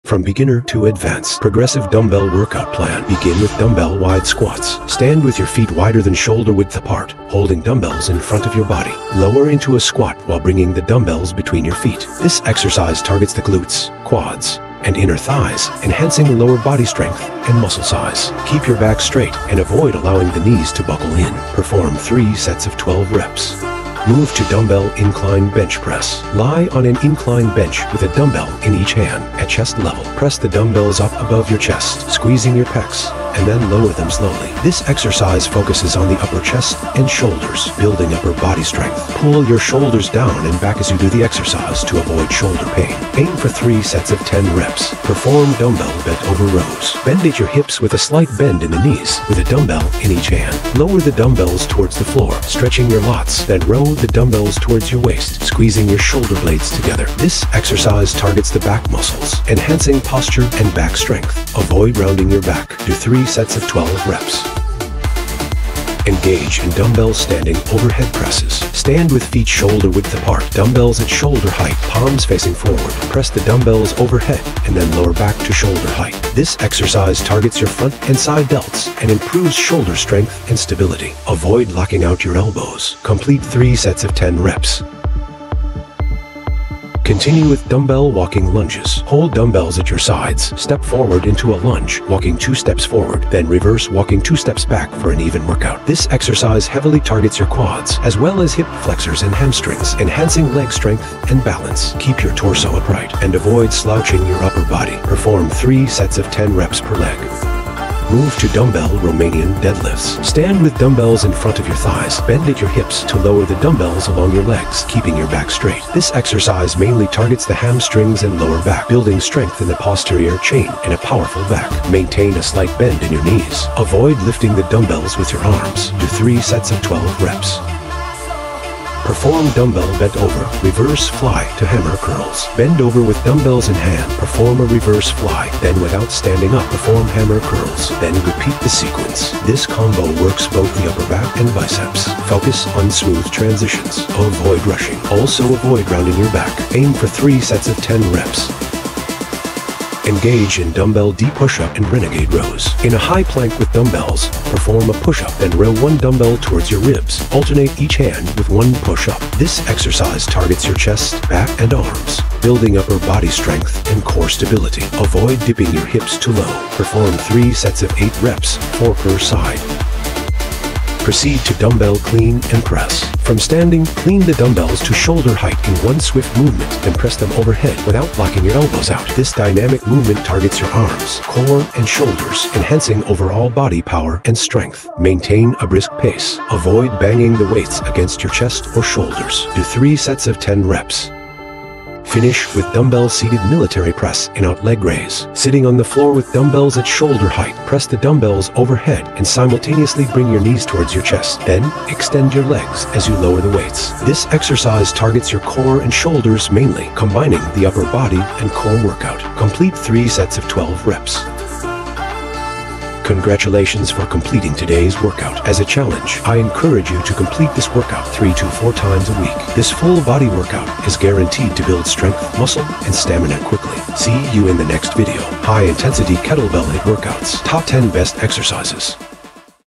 From Beginner to Advanced: Progressive Dumbbell Workout Plan!
Timeline/ Chapters:
00:00 - 00:02 - Intro
00:02 - 00:23 - Dumbbell Wide Squats
00:23 - 00:44 - Dumbbell Incline Bench Press
00:44 - 01:05 - Dumbbell Bent Over Row
01:05 - 01:26 - Dumbbell Standing Overhead Press
01:26 - 01:47 - Dumbbell Lunges
01:47 - 02:08 - Dumbbell Romanian Deadlifts
02:08 - 02:29 - Dumbbell Bent Over Reverse Fly to Hammer Curls
02:29 - 02:51 - Dumbbell Deep Push Up and Renegade Row
02:51 - 03:12 - Dumbbell Clean and Press
03:12 - 03:33 - Dumbbell Seated Military Press In Out Leg Raise
03:33 - 03:50 - Outro

Welcome to "From Beginner to Advanced: Progressive Dumbbell Workout Plan!" — a comprehensive workout series designed to help you build strength, improve endurance, and enhance overall fitness through a structured progression of dumbbell exercises. This workout plan is perfect for anyone looking to start their fitness journey with basic movements and gradually advance to more complex exercises, effectively transforming their physique and boosting their health.

Exercises Breakdown:

Dumbbell Wide Squats: Begin with a foundational lower-body exercise to build strength in your quads, hamstrings, and glutes.

Dumbbell Incline Bench Press: Progress to targeting your upper chest and shoulders, enhancing upper body strength with an incline angle.

Dumbbell Bent Over Row: Strengthen your back and improve posture with this essential upper body exercise.

Dumbbell Standing Overhead Press: Build powerful shoulders and arms by pressing dumbbells overhead, focusing on form and stability.

Dumbbell Lunges: Increase lower body strength and balance with lunges, a crucial exercise for functional fitness.

Dumbbell Romanian Deadlifts: Enhance hamstring and lower back strength, crucial for overall body lifting capabilities.

Dumbbell Bent Over Reverse Fly to Hammer Curls: Combine two exercises to work on your upper back and biceps, increasing upper body conditioning.

Dumbbell Deep Push Up and Renegade Row: Integrate a push-up with a row to challenge your chest, back, and core simultaneously.

Dumbbell Clean and Press: Develop full-body power and coordination with this compound movement, preparing you for advanced lifting techniques.

Dumbbell Seated Military Press In Out Leg Raise: Combine upper body pressing with core activation for a challenging multi-task exercise.

This workout is ideal for individuals at any fitness level, offering a structured plan that evolves as you grow stronger and more confident in your abilities. Follow along to see noticeable improvements in muscle tone, strength, and overall fitness as you progress from beginner to advanced levels!

Progressive Workout, Dumbbell Training, Full Body Exercise, Strength Building, Home Fitness, Beginner to Advanced, Muscle Toning, Fitness Plan.